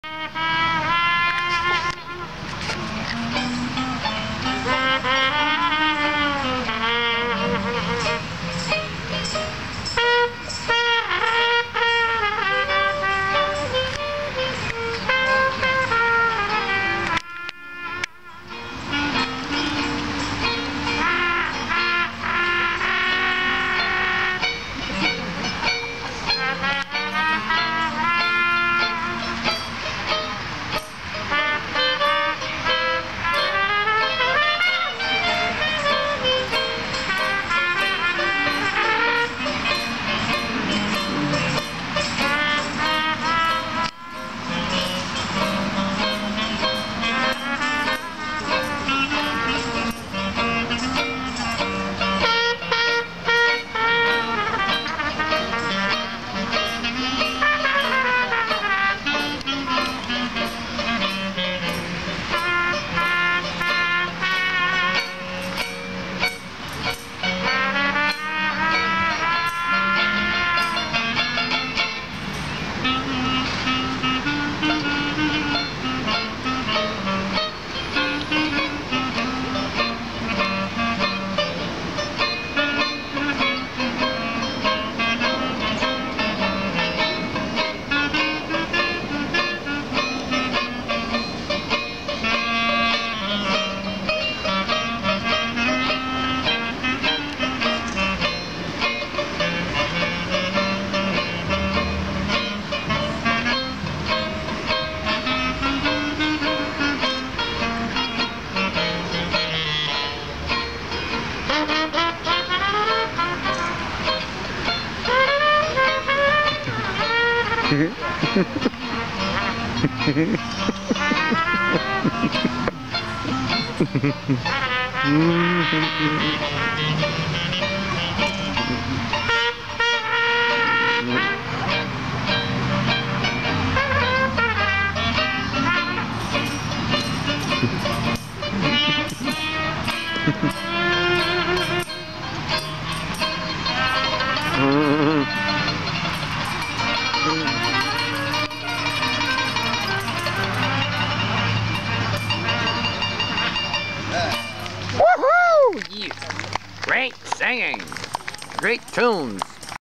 New Orleans Connection concert at Norton Public Library- Tuesday July 29, 2014 (2/7)
This is part 2 of the New Orleans connection concert at Norton Public Library this evening